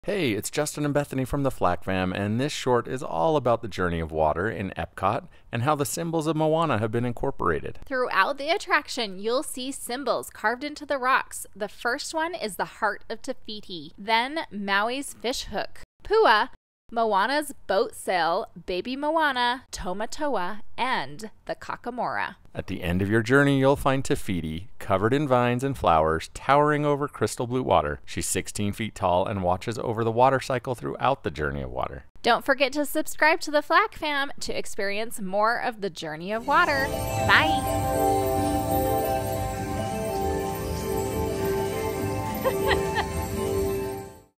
Journey of Water Short #3
This is the third short in a series of six for the Moana inspired Journey of Water in EPCOT.